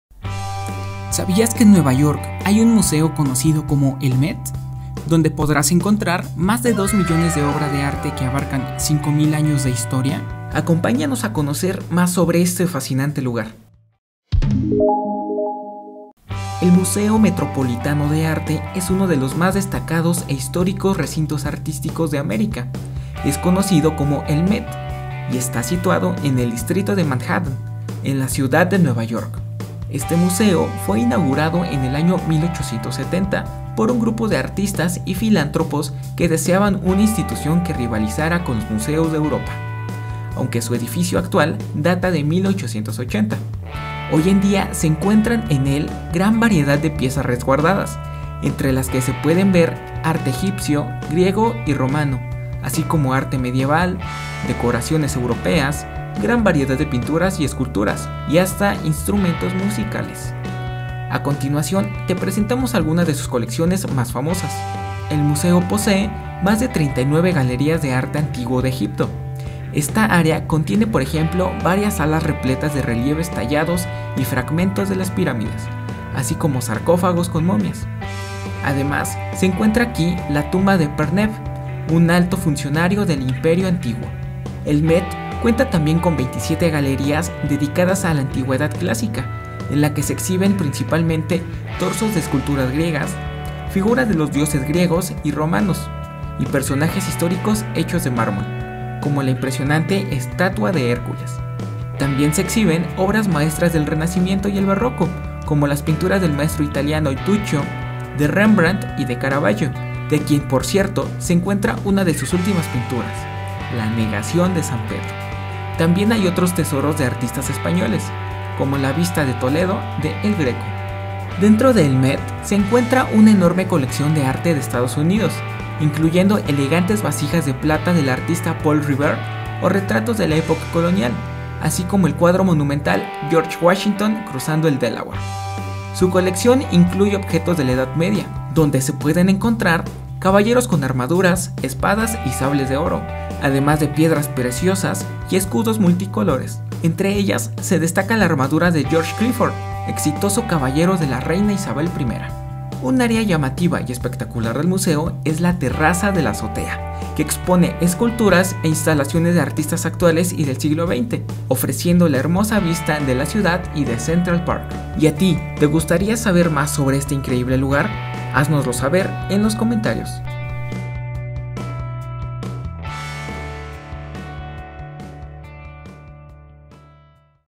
Inauguración del Metropolitan Museum of Art en Nueva York
El 20 de febrero de 1872 el Metropolitan Museum of Art conocido como MET abrió sus puertas al público en Nueva York. Ubicado en las cercanías de Central Park, es uno de los museos más grandes y completos del mundo; con más de dos millones de obras que abarcan 5.000 años de historia ¿Nos acompañas a conocerlo?